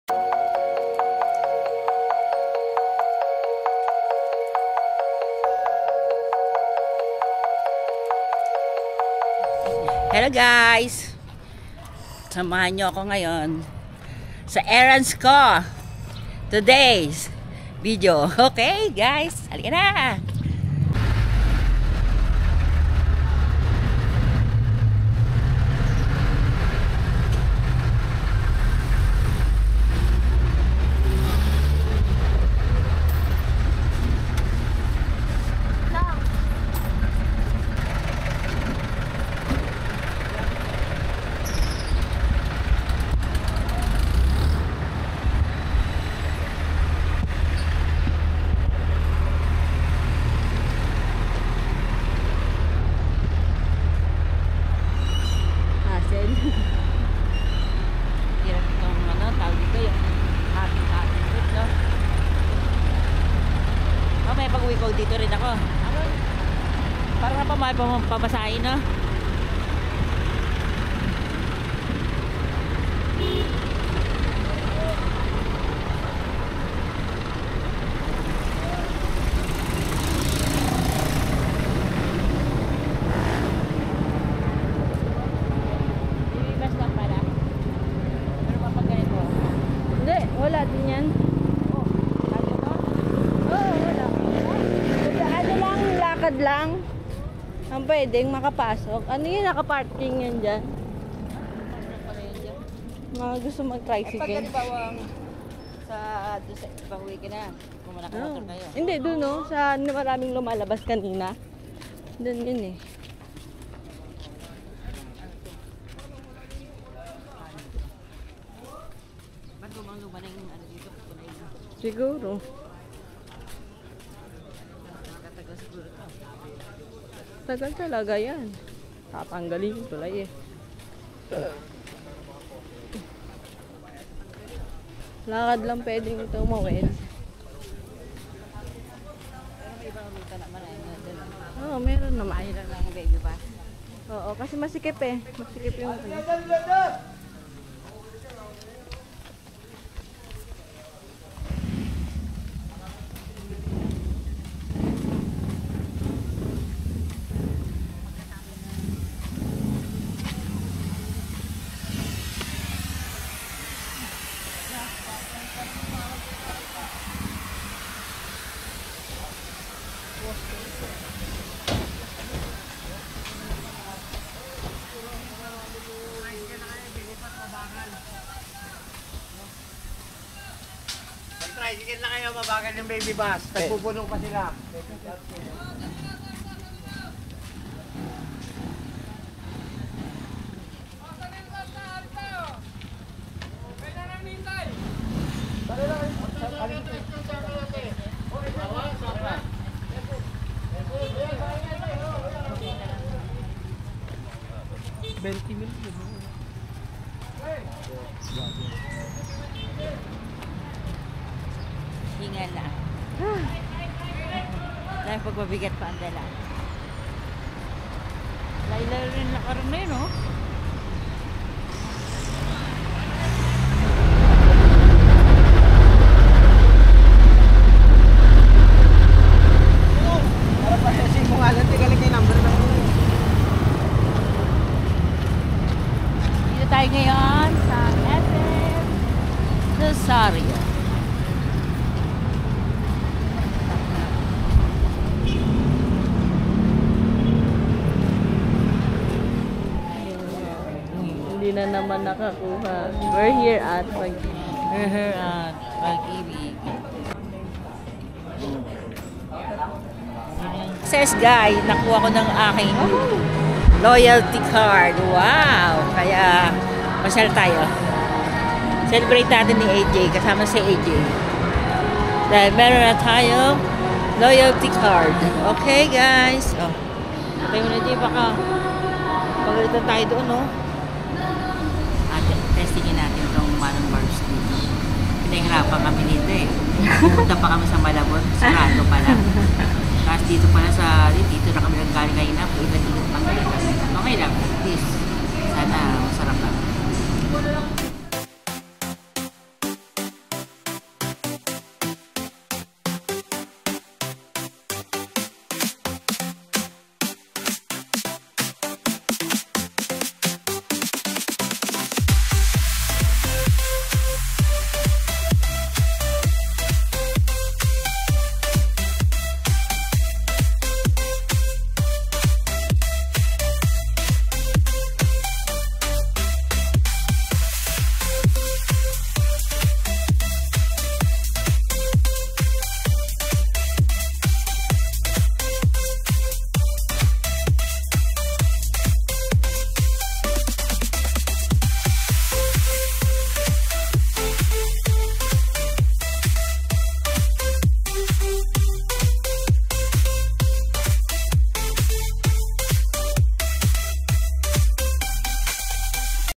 MP2 PAGIBIG SAVINGS WITH MANONG BARBS 🩵
DEPOSIT IN MY MP2 SAVING AFTER LUNCH OUT IN MANONG BARB RESTAURANT 🩵🩵

EDEN DA KANUNAL 🌹🌹